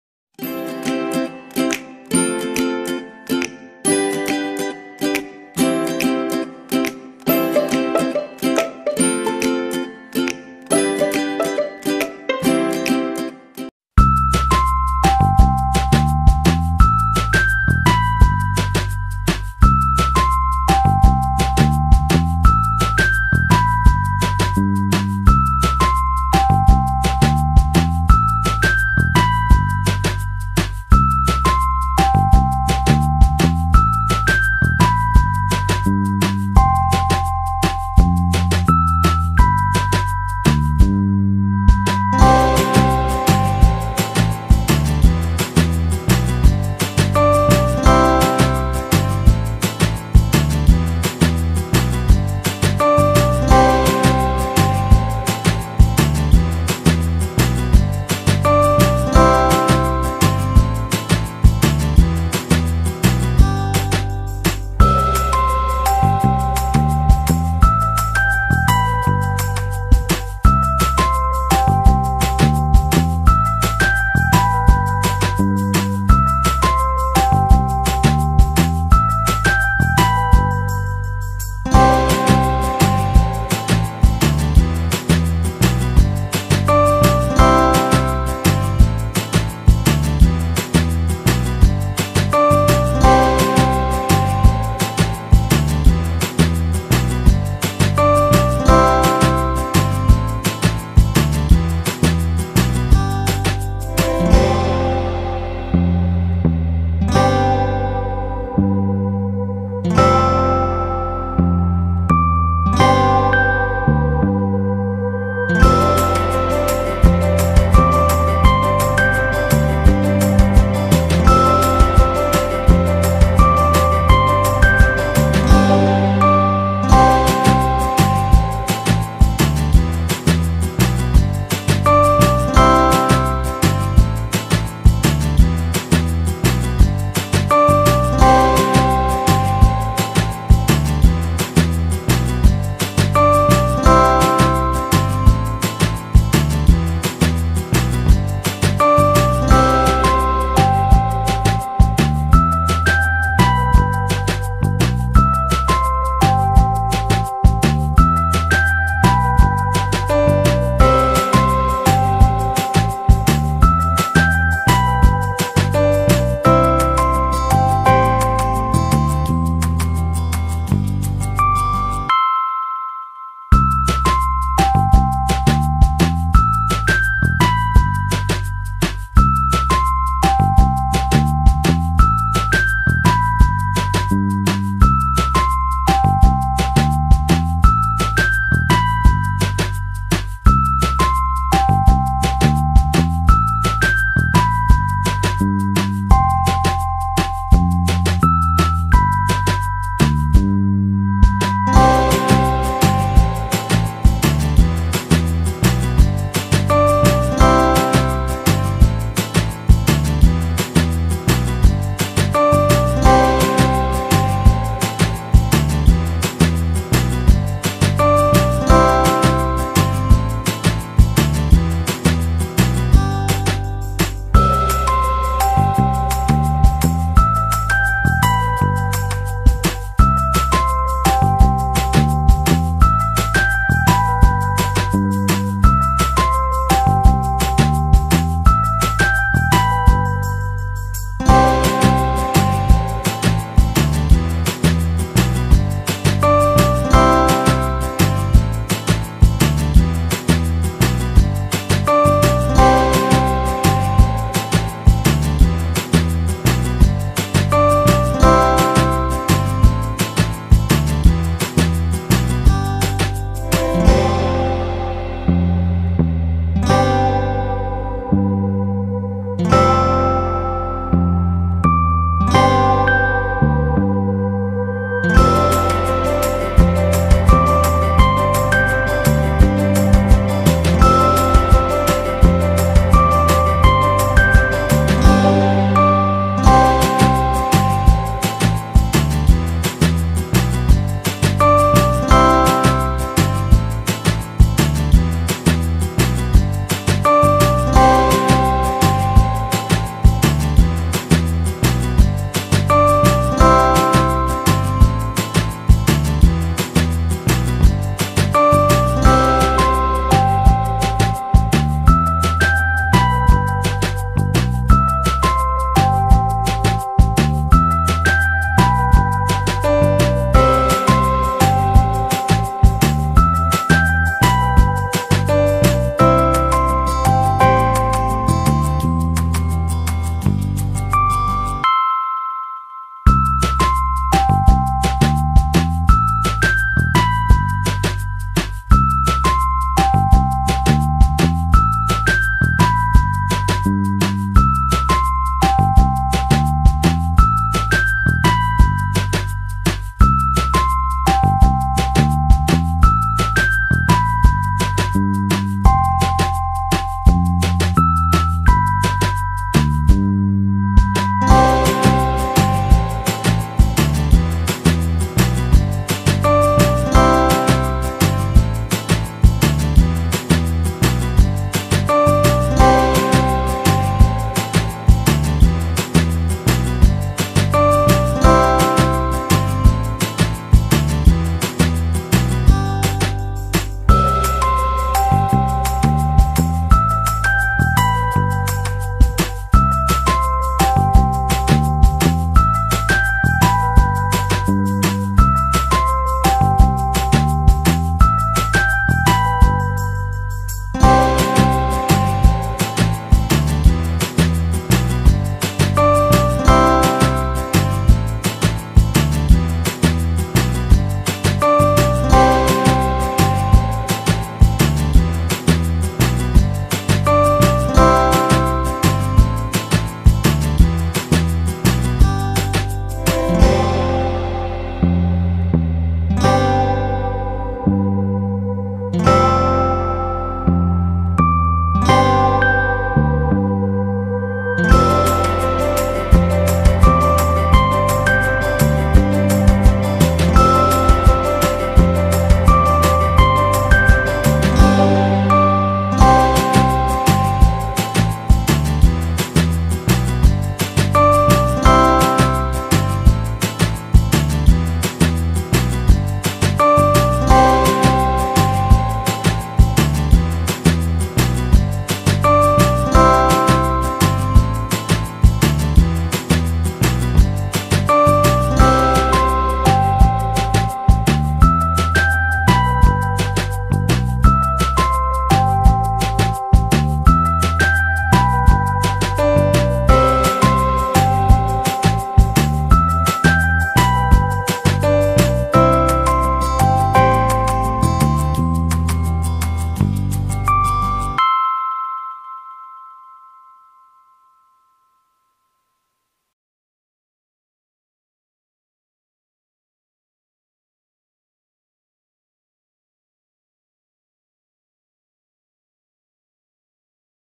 Wall decor/using waste materials/axe brand oil bottles(mini bottles)
This is a simple decor using the bottles which we use to throw often. Hope you all like the video